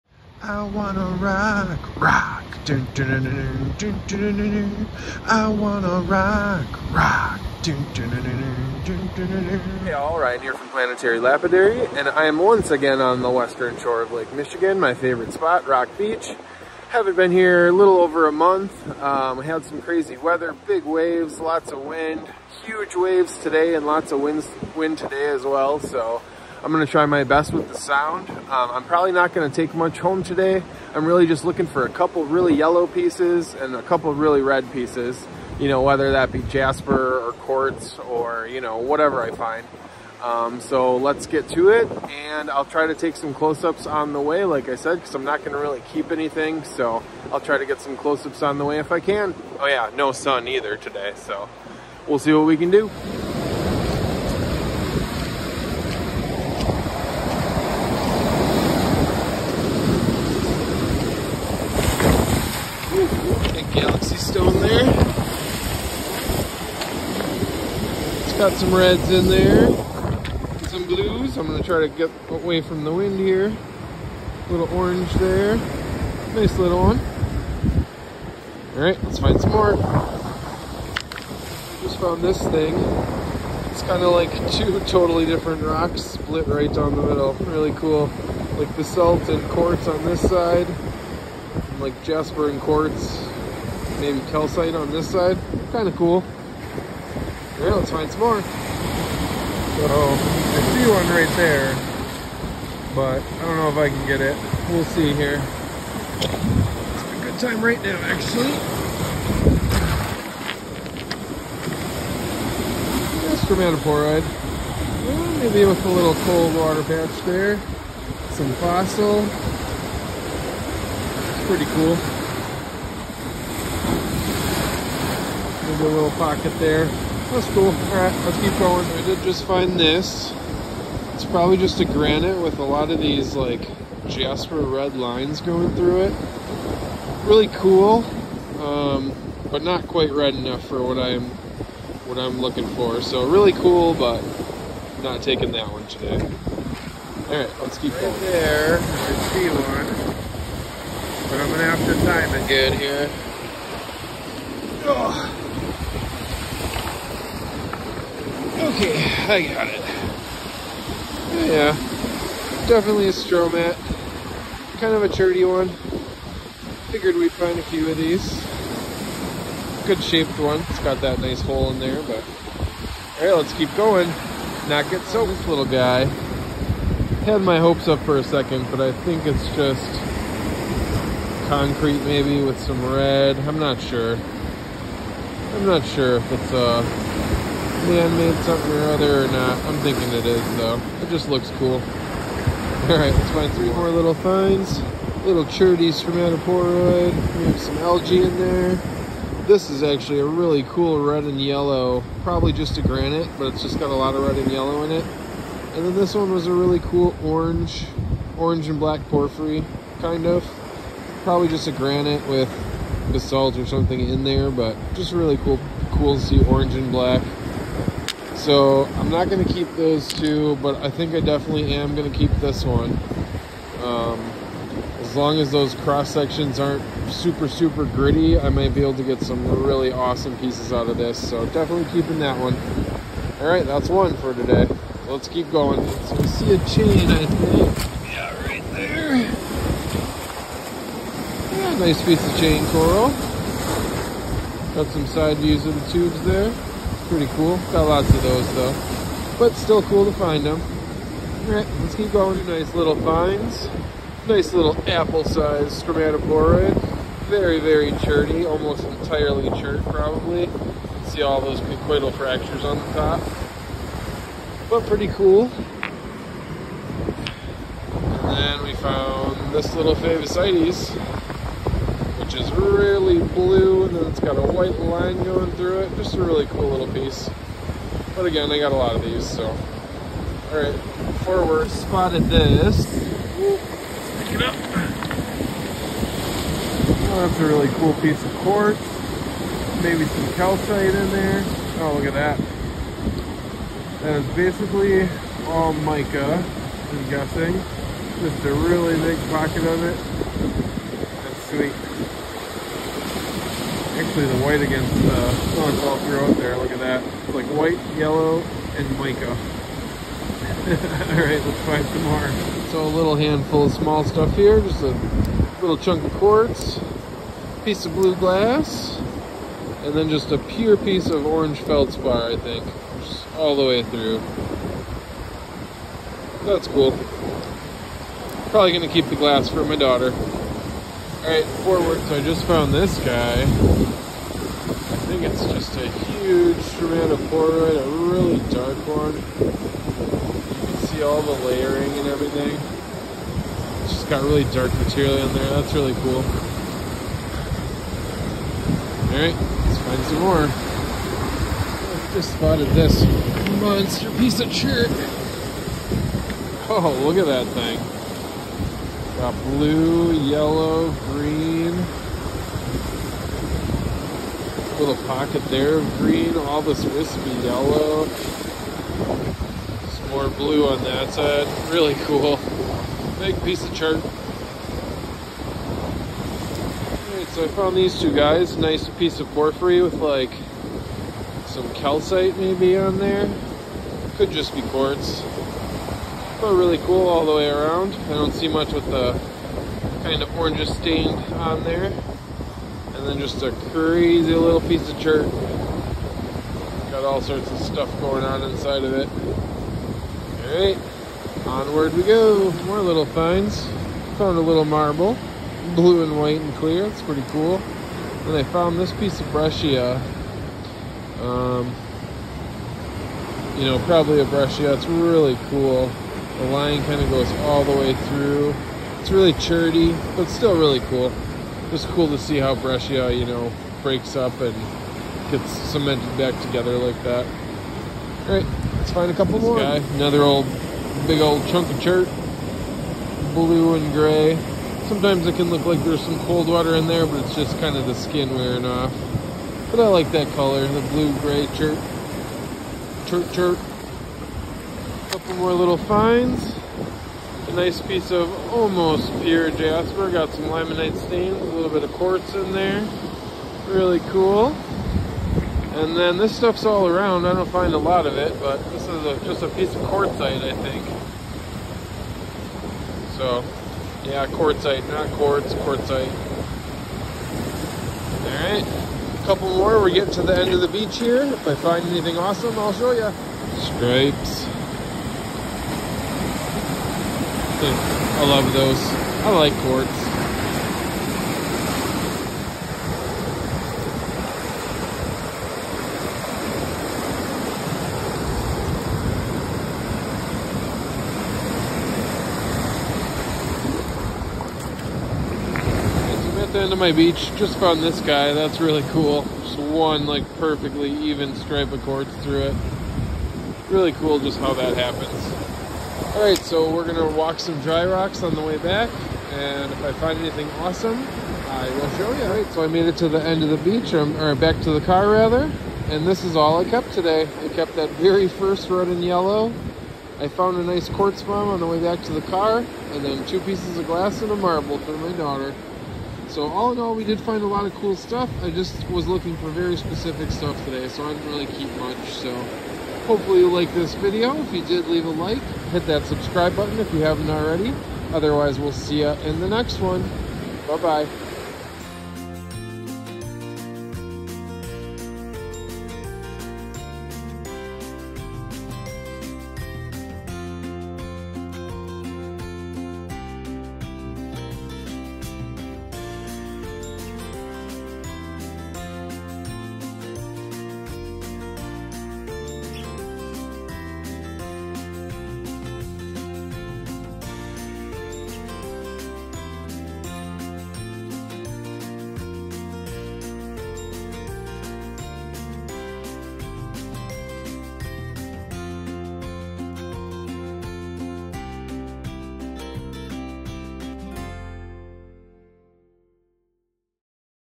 Lake Michigan Rock Hunt | Fall Rockhounding!
A nice hunt on the western shore of Lake Michigan!